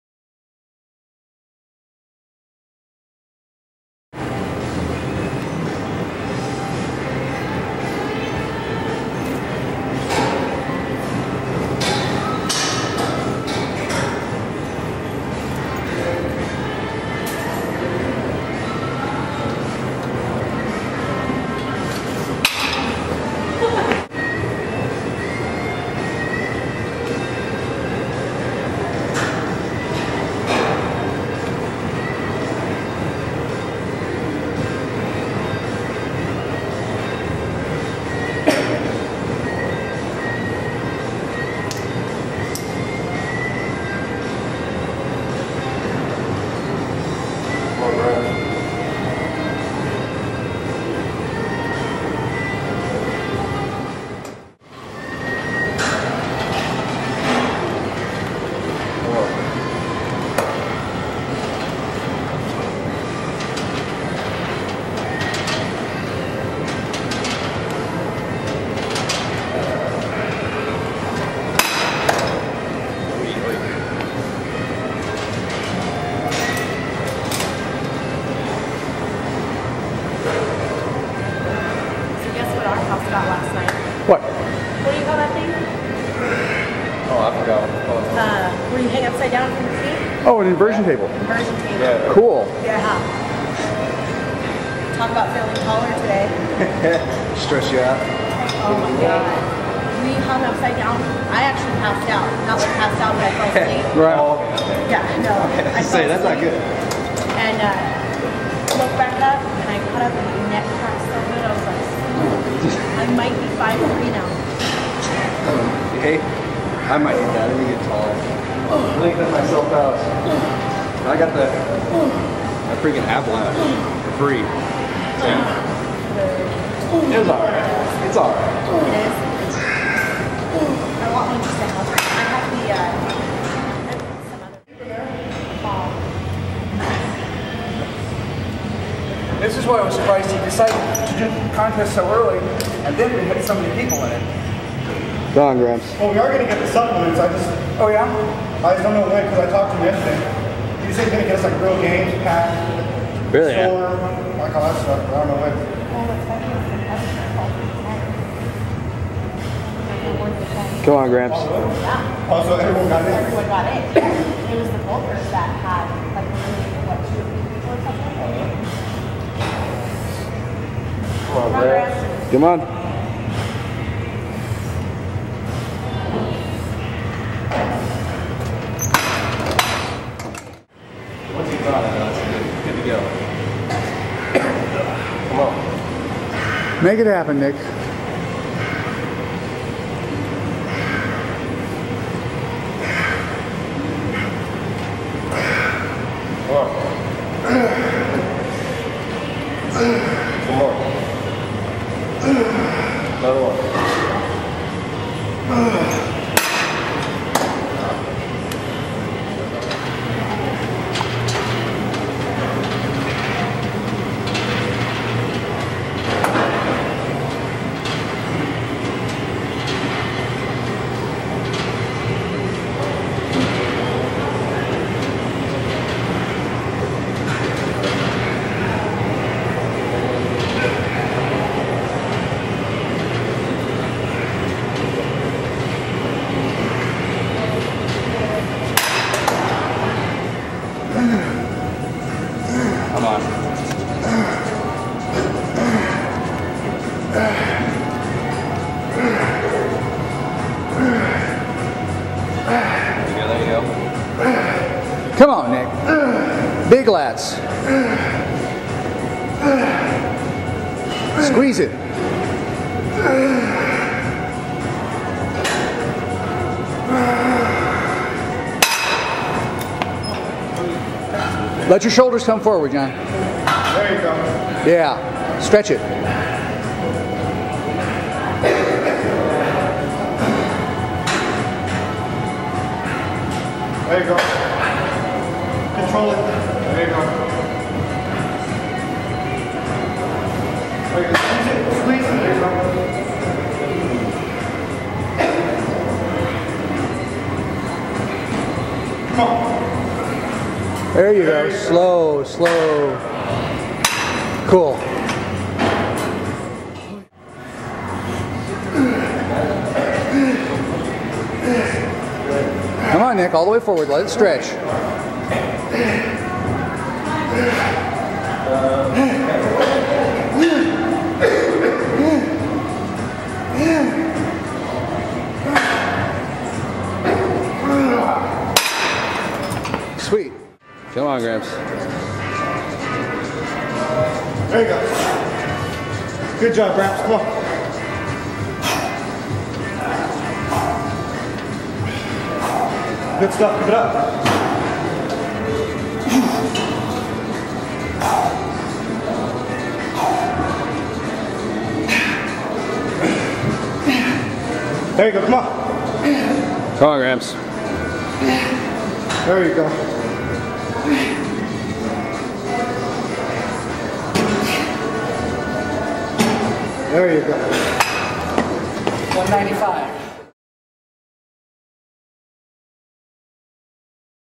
TempleGym11-27-09 Pulleys.mp4
5 Animals in search of wider, bigger backs invade Temple Gym in Riverside, Calfornia.  The two Victors from the first "Iron Contest" on the Animal Forum, and two contestants in Round Two, along with a competitive motocross rider we picked up along the way.  We warmed up with pulldowns and low pulley rows.